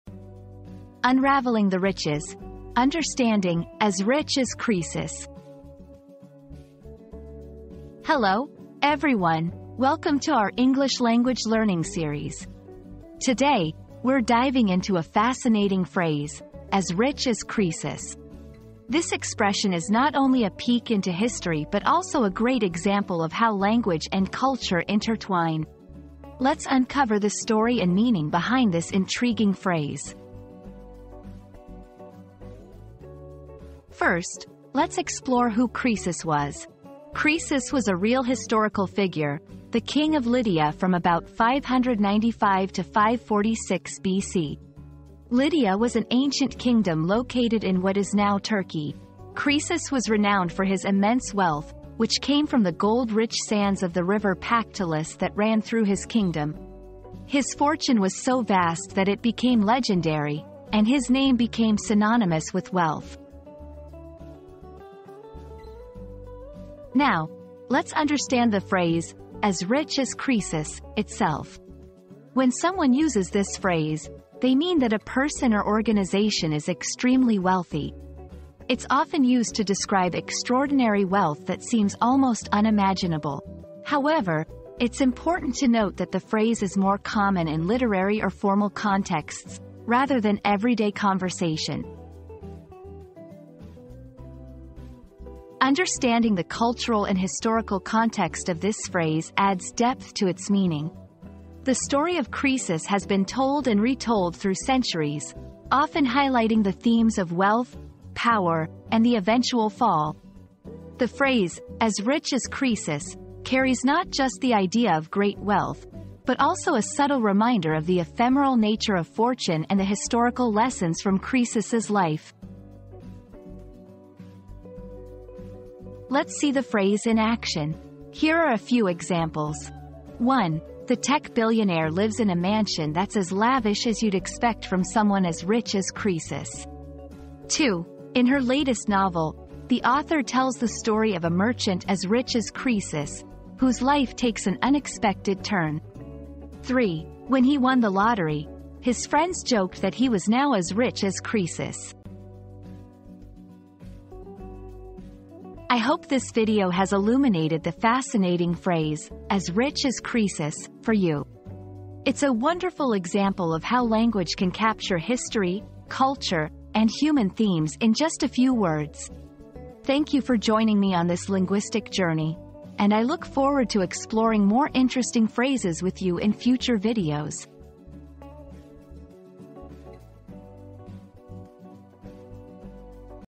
Unraveling the Riches: Understanding "As Rich as Croesus"
Unraveling the Riches: Understanding 'As Rich as Croesus' • Delve into the intriguing origins and meaning behind the popular phrase 'As Rich as Croesus.' Discover the fascinating story of Croesus, the ancient king known for his immense wealth and unravel the timeless allure of opulence and prosperity.

00:00 • Introduction - Unraveling the Riches: Understanding "As Rich as Croesus"
00:33 • Who Was Croesus?
01:06 • The Phrase's Meaning and Usage
01:35 • Cultural and Historical Context
02:05 • Examples in Sentences